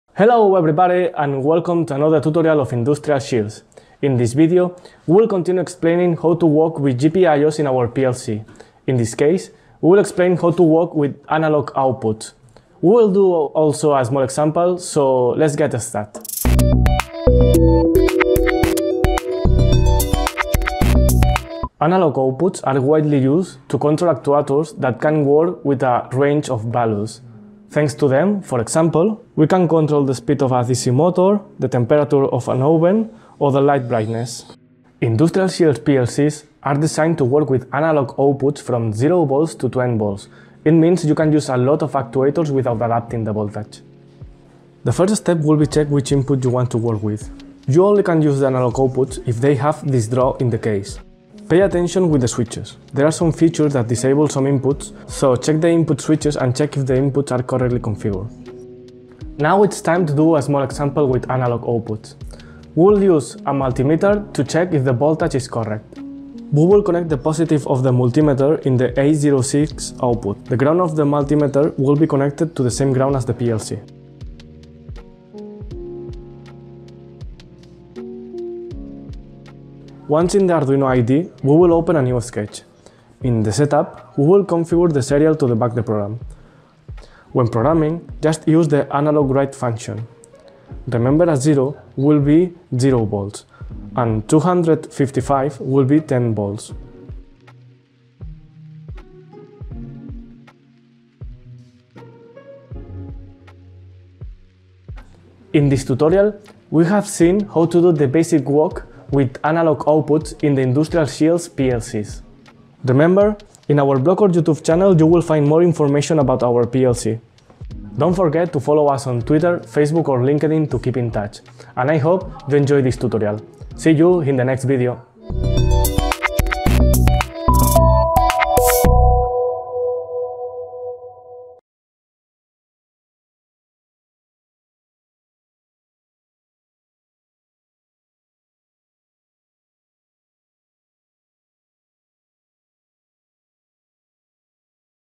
Basics: Analog outputs in Arduino PLC
This video explains how to work with analog outputs in an Arduino PLC.